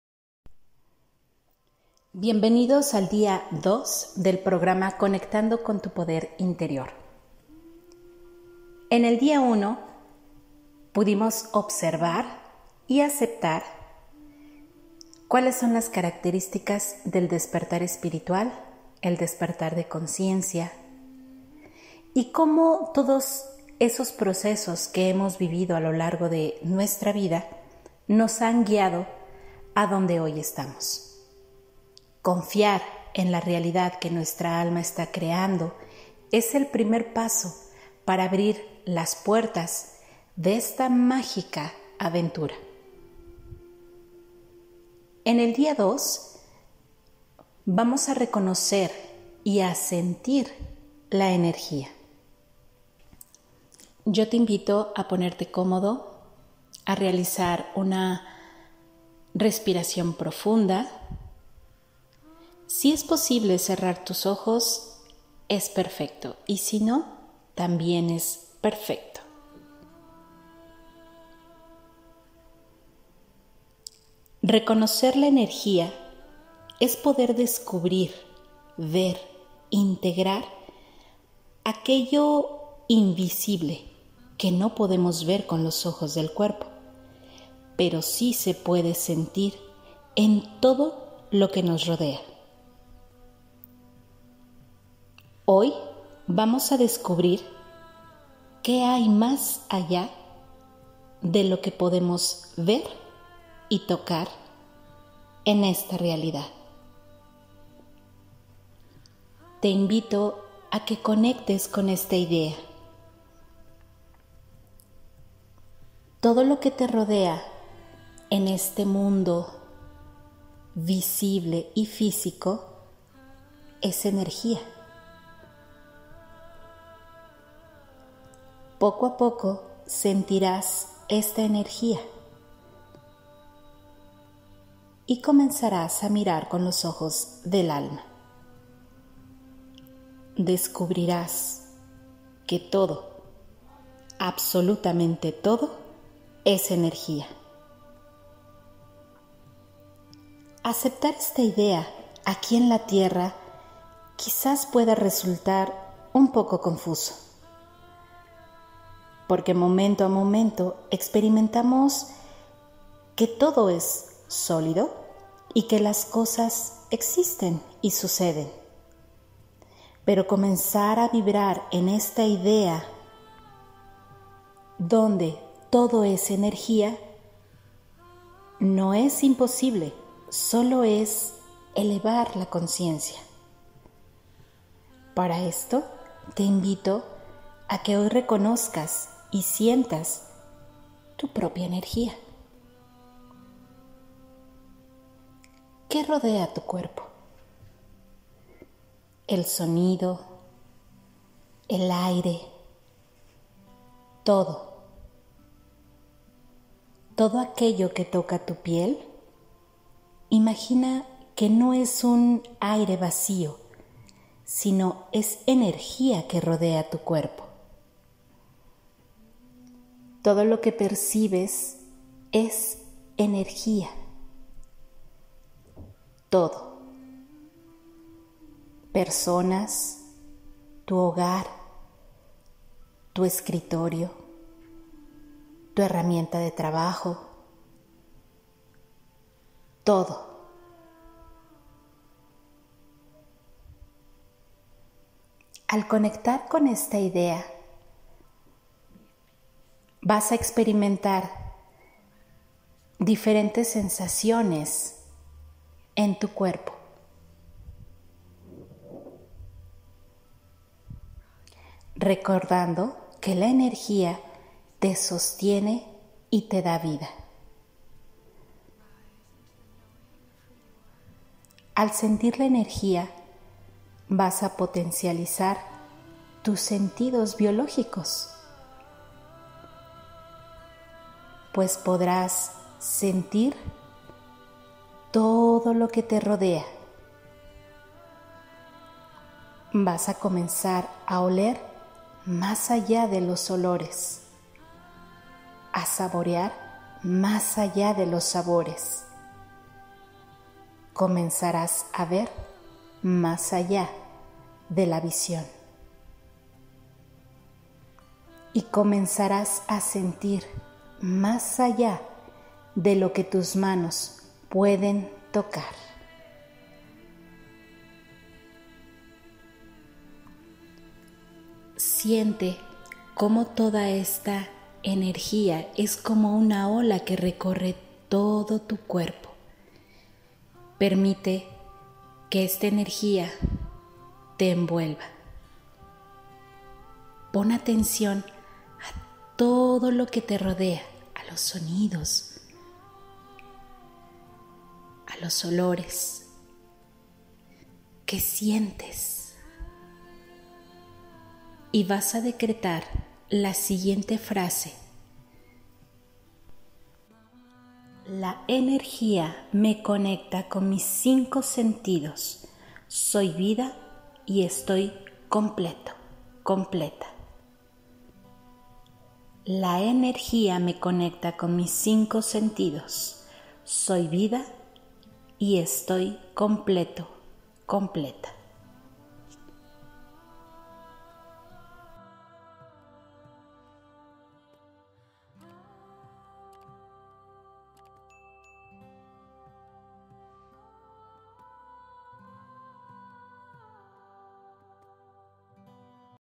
💥2 CONECTANDO CON TU PODER INTERIOR ✨ RECONOCE LA ENERGÍA 💠
Bienvenido al Reto 11 días para Conectar con tu Poder Interior.
A lo largo de estos días aceptarás, sentirás y sobre todo en conciencia conectarás con una energía superior que te guiará con tu PODER INTERIOR.
La vida se manifiesta y es perfecta!

Bienvenido a esta profunda aventura. Nada es casualidad ya estas listo para escuchar esto.

Gracias por tu tiempo si esta sabiduría te resuena comparte, regalanos un like. Gracias, gracias, gracias.

Si gustas hacer alguna donación comparto el link para que lo puedas realizar. Gracias, gracias, gracias.